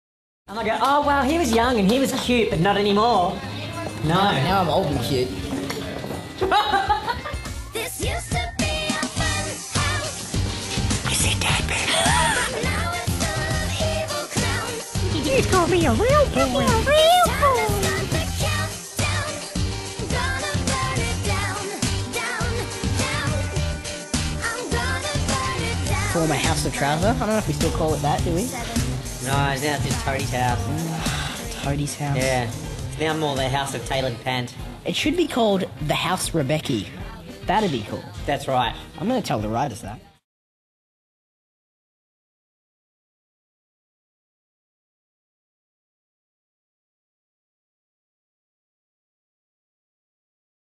ryan moloney + eve morey + morgan baker | ...the house rebecchi
MY FIRST TEAM REBECCHI VIDEO :)))))
I LOVE IT SOOOOOOOOOOOOOOOOOOOOOO MUCH. AND WHAT MAKES IT BETTER; IT IS A REAL-LIFE TEAM REBECCHI VIDEO FOCUSED ON RYAN/EVE/MORGAN! :D

As soon as I saw Morgan's BTS video of his departure and got over my amazement that it took so just to get a proper Team Rebecchi scenes. I just knew that I had to make this, it wasn't even a decision, just like an instinct of something. something had to be done to acknowledge this beautiful OT3 and I know this video isn't much but...I hope you all like it, I needed to do something and hopefully will be able to make more videos about the incredible Team Rebecchi soon.

Also. Let me know if you want to see more 'real-life' Neighbours videos from me, like, with videos focused on actors, the friendships seen between actors etc. If yes; let me know who you want to see in one of these videos.